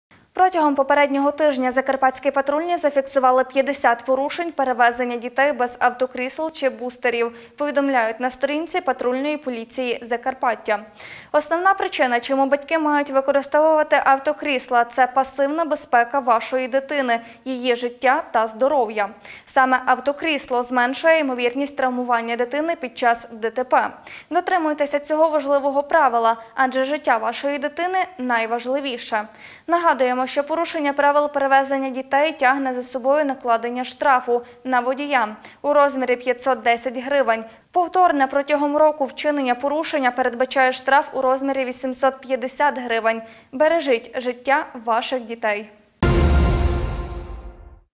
Порушення при перевезенні дітей
Протягом попереднього тижня закарпатські патрульні зафіксували 50 порушень перевезення дітей без автокрісел чи бустерів, повідомляють на сторінці патрульної поліції Закарпаття. Основна причина, чому батьки мають використовувати автокрісла — це пасивна безпека вашої дитини, її життя та здоров’я.